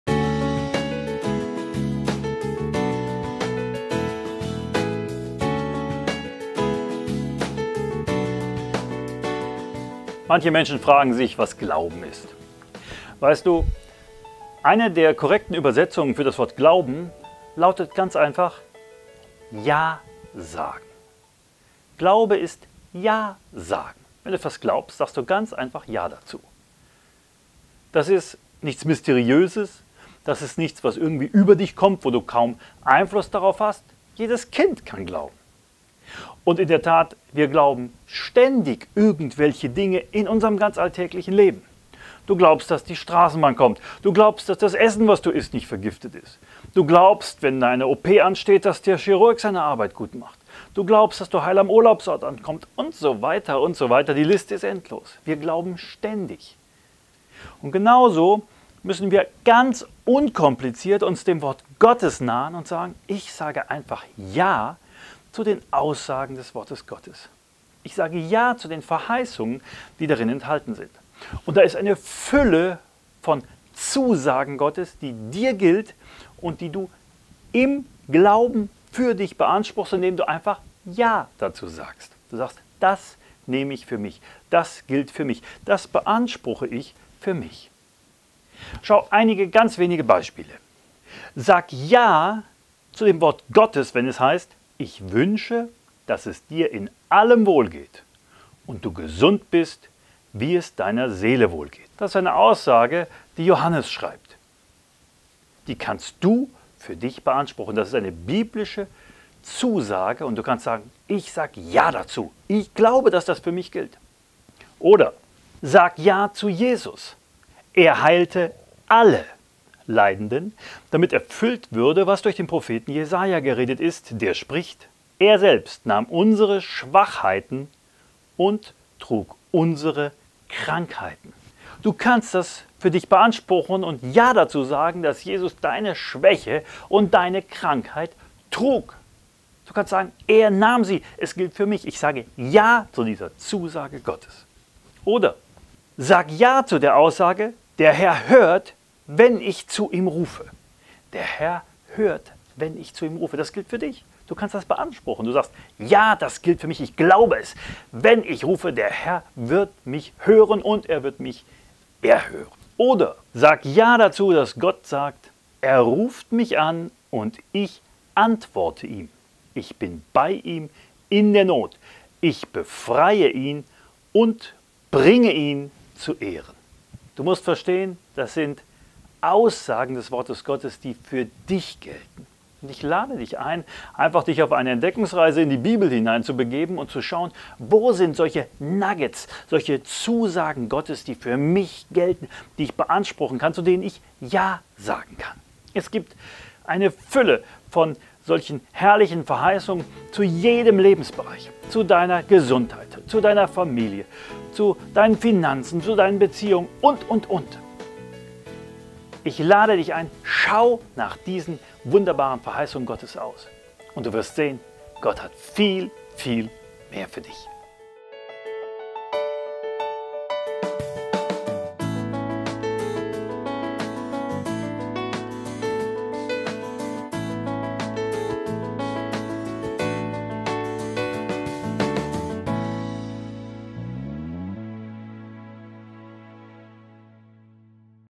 Glaube heißt „Ja“ sagen
Es gibt Leute, die sagen: „Ich kann nicht glauben.“ Doch das ist Unsinn, denn eine korrekte Übersetzung des Wortes „Glauben“ lautet ganz einfach: „Ja sagen!“
Wenn du zu etwas „Ja“ sagst, dann glaubst du es. Das kann jedes Kind. Entscheide dich „Ja“ zu dem zu sagen, was Gott sagt. Fang heute damit an und dein Leben wird sich dramatisch verändern.

Glaube ist nichts Mysteriöses, Unkontrollierbares, das irgendwie über dich kommt und auf das du keinen Einfluss hast. Du kannst dich heute entscheiden zu glauben. Sag einfach: „Ja!“

Jeder von uns glaubt ständig. Wir könnten gar nicht leben, ohne zu glauben. Du glaubst zum Beispiel, dass der Zug oder die Straßenbahn kommt, dass das Essen nicht vergiftet ist, dass du heil am Urlaubsort ankommst, dass der Chirurg  seine Arbeit richtig machen wird usw. Die Liste ist endlos.

Entscheide dich, ab jetzt zu dem „Ja“ zu sagen, was Gott sagt.

Sag ja zu:
Geliebter, ich wünsche, dass es dir in allem wohlgeht und du gesund bist, wie es deiner Seele wohlgeht.
3.Johannes 1,2

Sag ja zu:
Er heilte alle Leidenden, damit erfüllt würde, was durch den Propheten Jesaja geredet ist, der spricht: Er selbst nahm unsere Schwachheiten und trug unsere Krankheiten.
Matthäus 8,16-17

Sag ja zu:
Der HERR hört, wenn ich zu ihm rufe
Psalm 4,4-6
Sag ja zu:
Er ruft mich an, und ich antworte ihm. Ich bin bei ihm in der Not. Ich befreie ihn und bringe ihn zu Ehren.
Psalm 91,15